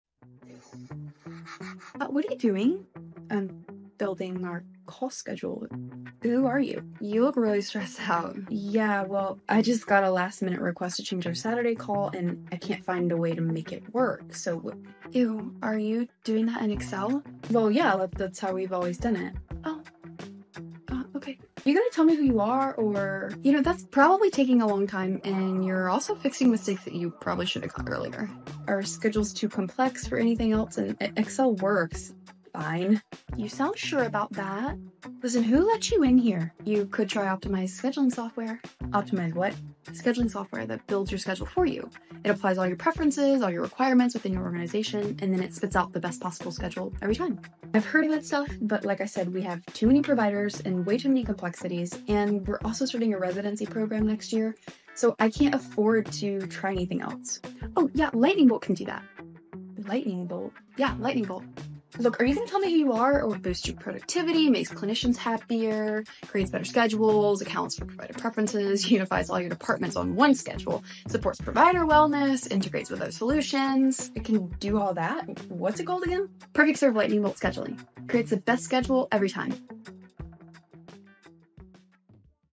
Using Manual Excel Sheets for Physician Schedules? PerfectServe Does It Better
Get your schedules to work for you.No matter how complex they seem, we got your back.